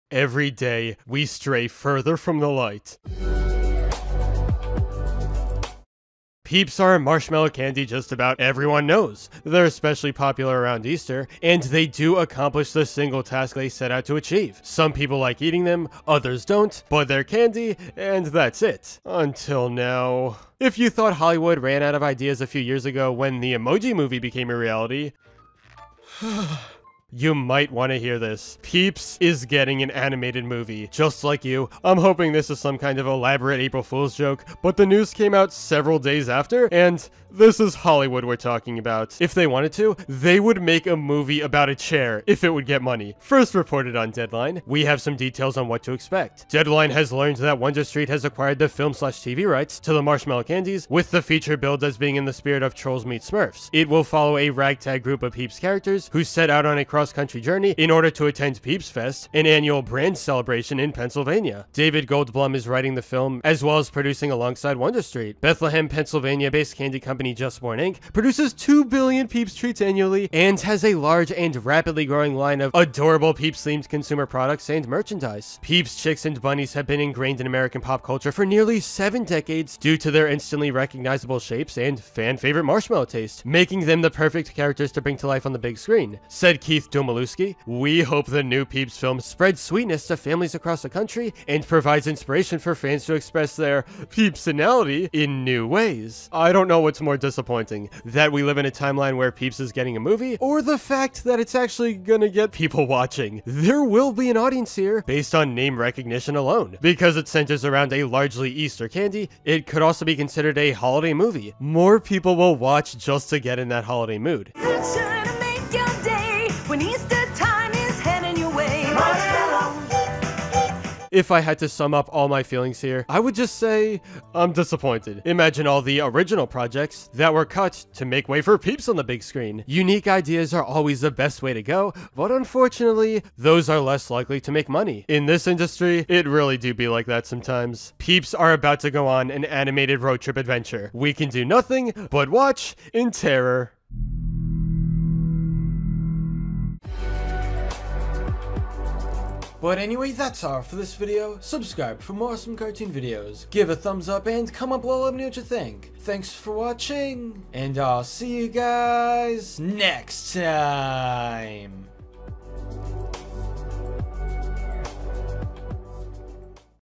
Peeps is Getting an Animated Movie
The Emoji Movie was only the beginning. Hollywood ran out of ideas long ago, which explains why a Peeps movie is officially happening. The Easter marshmallow candy has been a holiday staple for years, only now jumping onto the big screen in a road trip adventure. This is not an April Fools joke.

Seen as a sort-of mix between The Smurfs and Trolls, the Peeps movie will follow lifelike candy characters on a brand new adventure of their own.

✭ SOCIAL MEDIA ✭